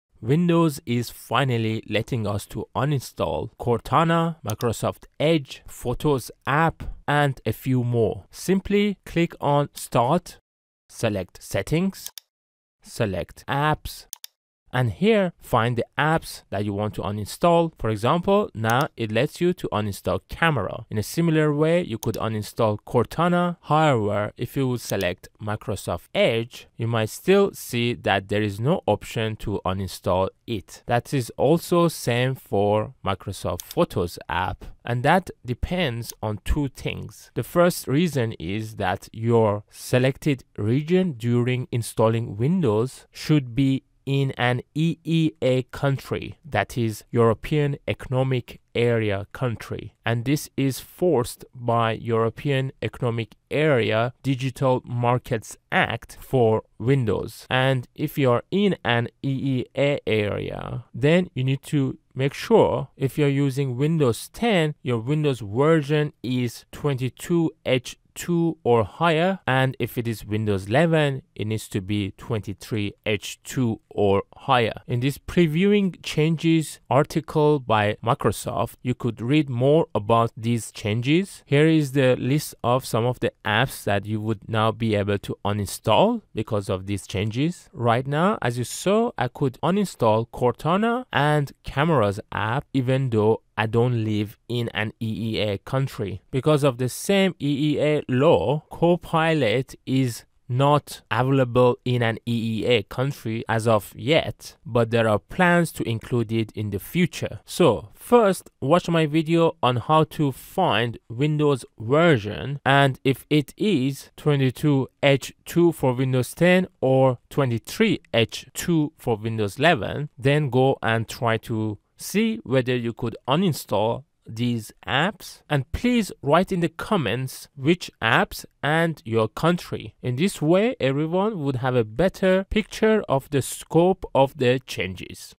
How to uninstall Edge, Photos, Cortana from Windows - LATEST UPDATE
Finally! In windows latest update you could now uninstall Microsoft Edge, Photos App, Cortana & Bing from Windows! Watch this short tutorial to learn how…

European Economic Area law called Digital Markets Act (DMA) will come into force on March 6th, 2024. Which means users should be able to uninstall any software, app, or services from their devices that they don't want. As it's an European Union law, it's possible that Microsoft wouldn't allow people from other regions to uninstall these apps. Nevertheless, it can make it too complicated for Microsoft to develop & maintain the various versions of Windows. So, hopefully, later everyone would be able to uninstall the apps that they don't use. Just like Apple was forced to change its lightning port to a USB-C port because of the European Union law.

Previewing changes in Windows to comply with the Digital Markets Act in the European Economic Area

So, to be able to uninstall these apps,  make sure your Window is at least updated to 22H2 for Windows 10 & 23H2 for Windows 11. Then, follow the steps above to see which one of these apps you could uninstall (Microsoft Edge, Photos App, Cortana & Bing). In the UK as of now, I was able to uninstall Cortana & Camera but wasn't able to uninstall Microsoft Edge or Photos App. Please mention in the comments, which apps you're able to uninstall & your country/region?